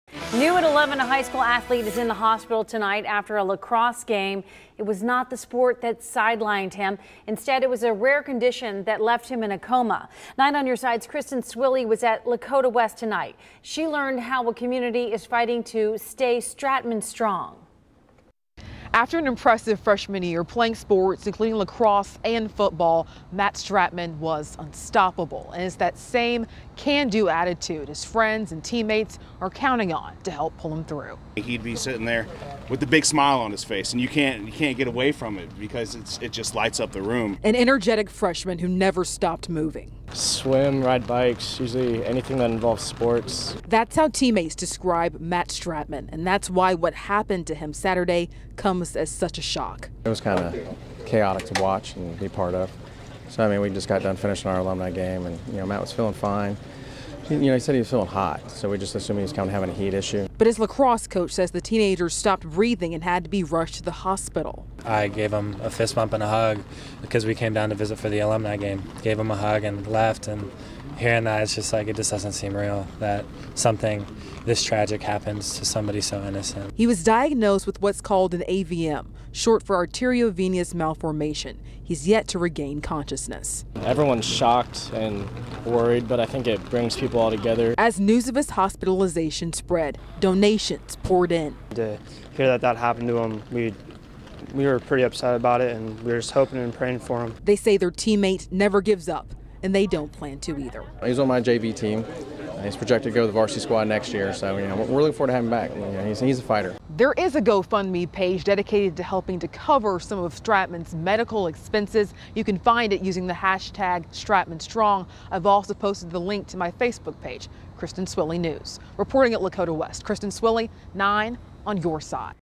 Lakota West 'praying' for comatose athlete
Lakota West lacrosse player Matt Stratman, whose freshman year athletic debut impressed coaches and classmates alike, seemed unstoppable until the moment he collapsed Saturday night.  Three days later, he still hadn't regained consciousness.